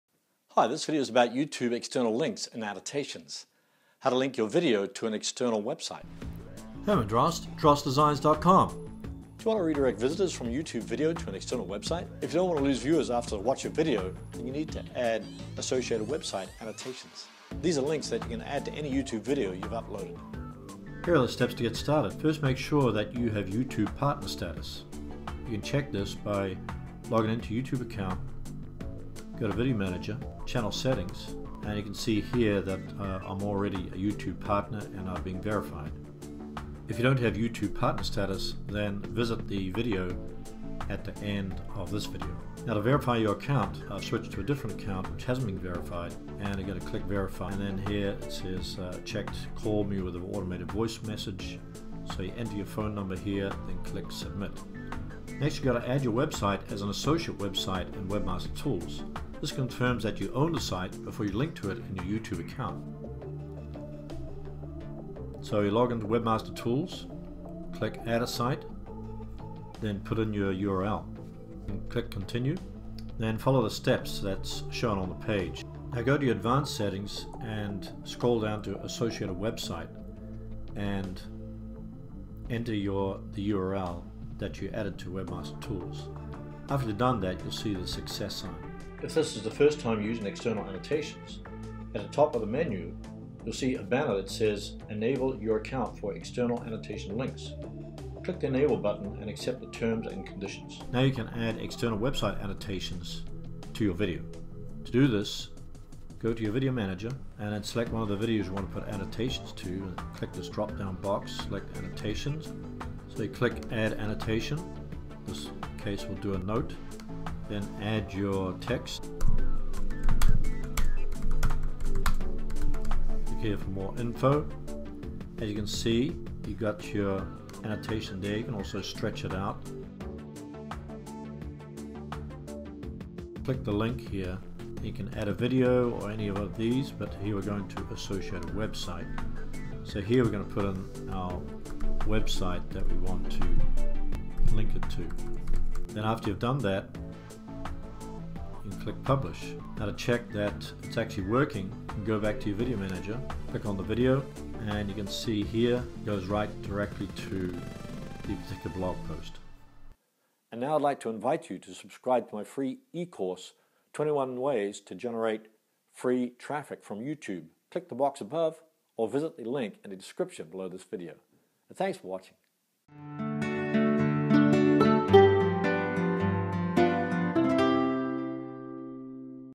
YouTube External Links In Annotations - How To Link Your Video To An External Website
YouTube External Links In Annotations - How To Link Your Video To An External

Do you want to redirect visitors from your YouTube video to an external website?

If you don't want to lose viewers after they've watched your video you need to add  "associated website annotations." These are links that you can add to any YouTube video you've uploaded.

Here are the steps to get started...

1. First make sure that you have YouTube partner status. You check this by logging into your YouTube account go to video manager, channel settings and you can see here that I'm already a YouTube partner and I have been verified.

If you don't have YouTube partner status then visit the video at the end of the above video.

2. Now to verify your account I've switched to a different account which hasn't been verified and I'm going to click verify and then it says here checked "call me with an automated voice message" so you enter your phone number here and then click "submit"

3. Next you've got to add your website as an associate website in webmaster tools. This confirms that you own the site before you link to it in your YouTube account.

4. So you login to webmaster tools

5. Click "add a site" then put in your URL

6. Click continue then follow the steps that's shown on the page

7. Now go to your advanced settings and scroll down to associated website and enter the URL that you added to webmaster tools. After you've done that you'll see the success sign.

If this is the first time you're using external annotations, at the top of the menu you'll see a banner that says "Enable your account for external annotation links."

Now you can add external website annotations to your video

To do this go to your video manager and then select one of the videos you want to put annotations to and click this drop down box select annotations so you click "add annotation" in this case we'll do a note then add your text "click here for more info"

As you can see  you've got  your  annotation there you can also stretch it out then you can click the link. You can add a video or any on the list but we're going to select "associated website" so here we're going to put in our website that we want to link it to
then after you've done that  click "publish"

To check that it is actually working go back to your video manager click on the video. It should go directly to your particular blog post (external website).

"21 ways to generate free traffic from YouTube"

Resources